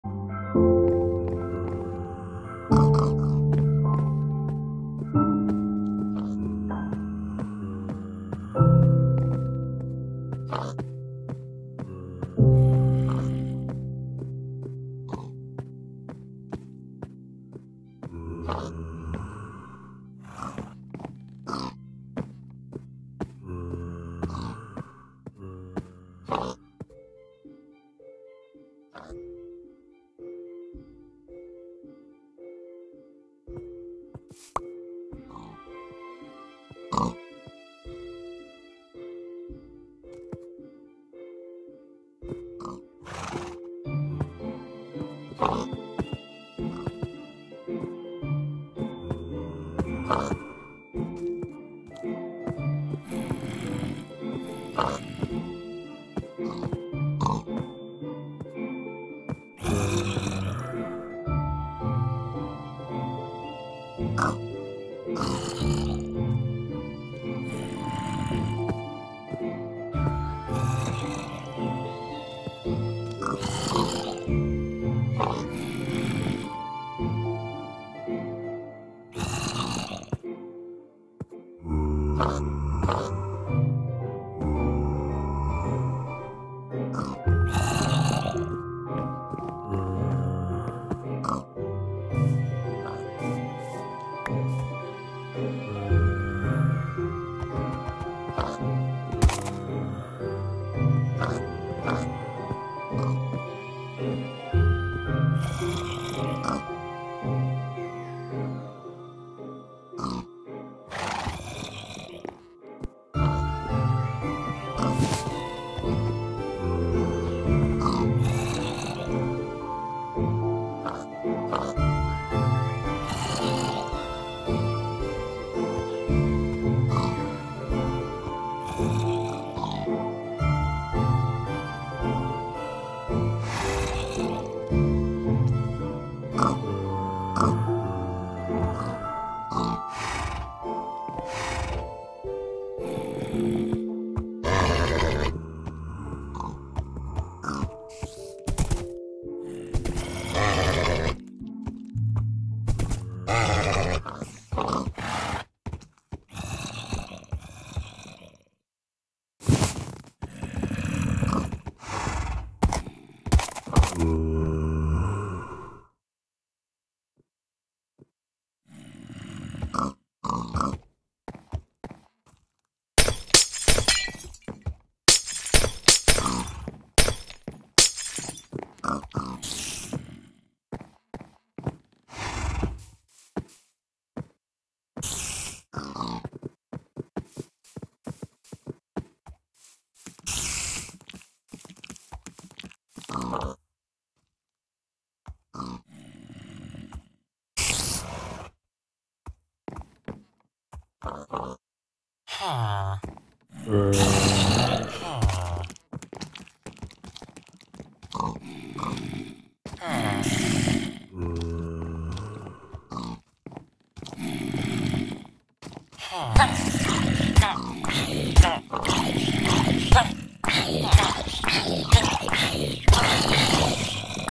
MC-96564
Mob can not normal walking(In situ the rotation) on the light sensor.
Riding a pig to make the pig kept jumping.